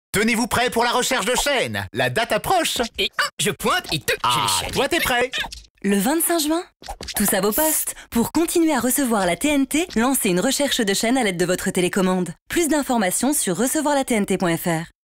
Isère et Savoie : changements de fréquences à venir ! (15 sec)
Le 25 juin 2019, les fréquences de la TNT changent pour l'Isère et la Savoie !